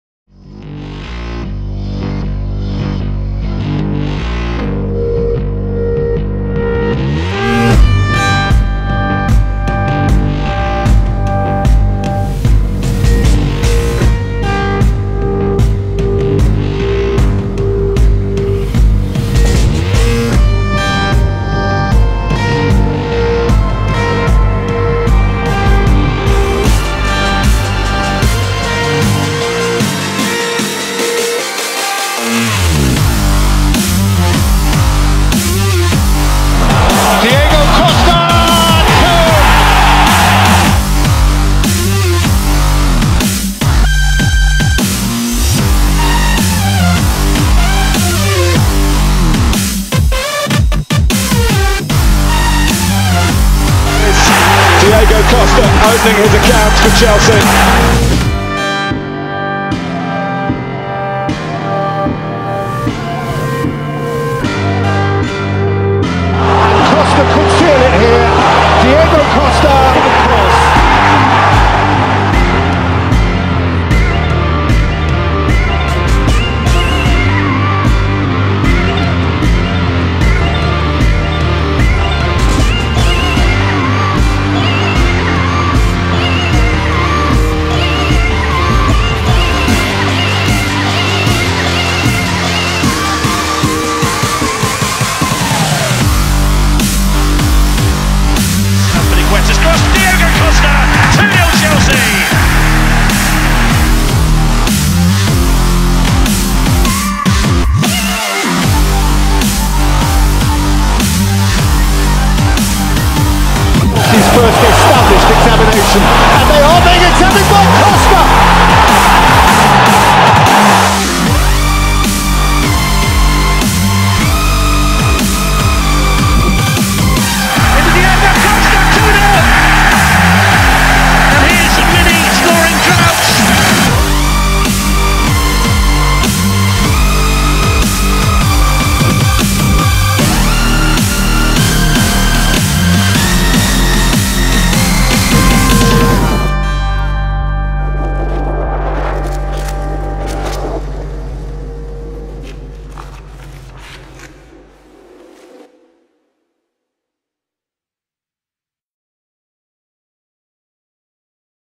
Diego Costa - The Guv'Nor
Some goals and skills by Diego Costa with Chelsea FC in the new season 2014/2015. Comment and like the video! Thanks.

Director: Cosimo Vanni
Release Date: January 2015
Genre: Sport